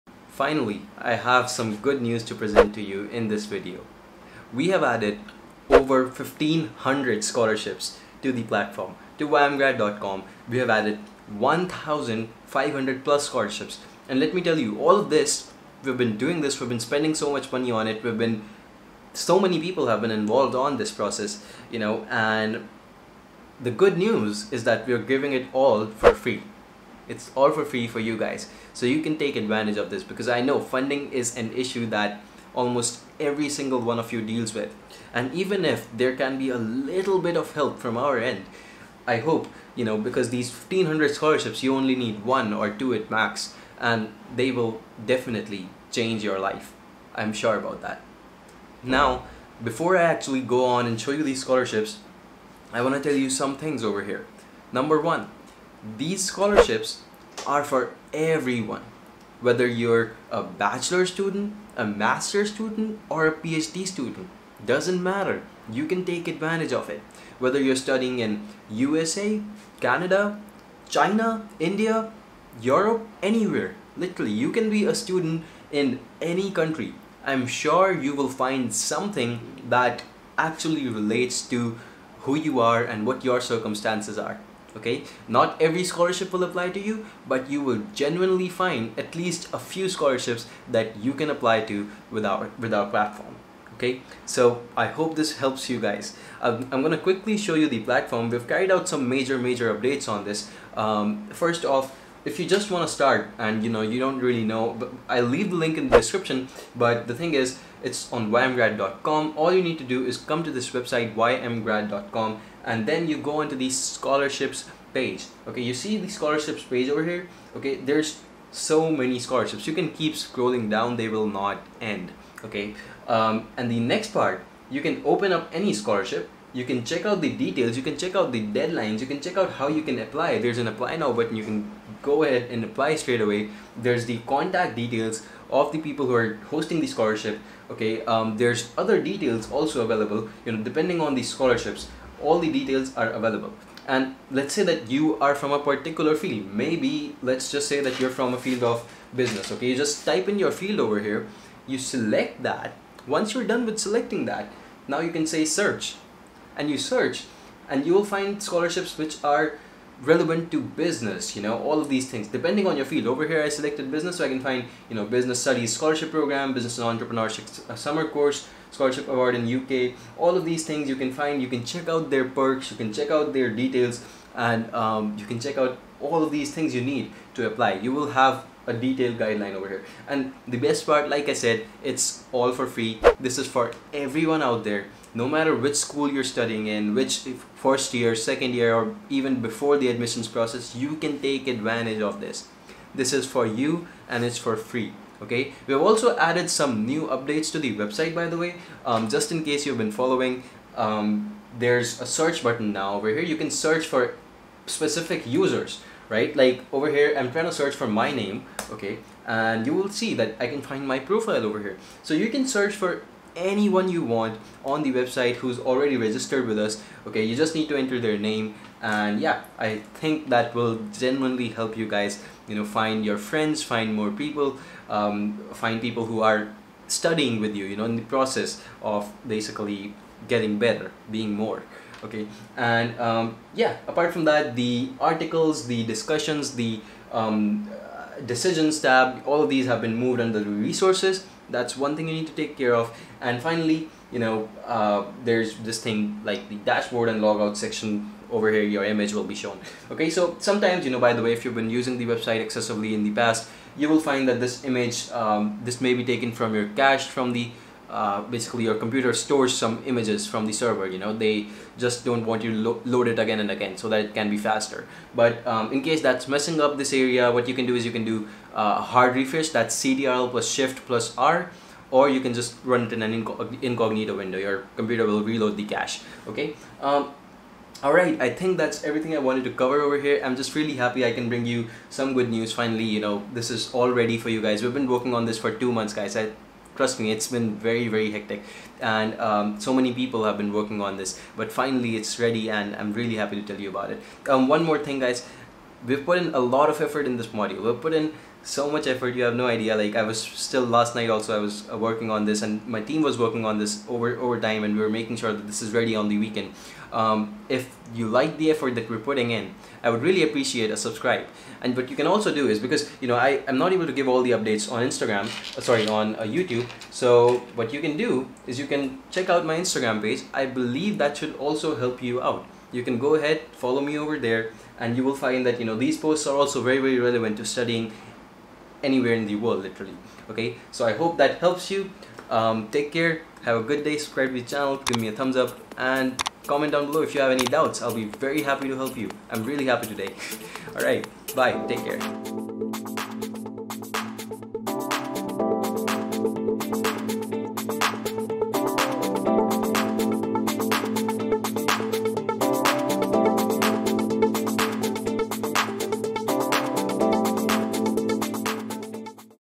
1500+ Bachelor's, Master's, and PhD Scholarships for FREE || Anywhere in the world (English)
Good news! We have successfully collated a list of 1500+ Bachelor's, Master's, and PhD scholarships for FREE! No tricks, no conditions. Funding is usually the biggest issue when it comes to students and their higher studies. Here, this is our attempt to give back. Find application deadlines, scholarship eligibility, application links, and everything else you need to make the best of your higher education. I guarantee you. No matter where you are, or who you are, if you are a student, there's a scholarship for you here.

Do you need help studying abroad, working abroad, or getting a visa,?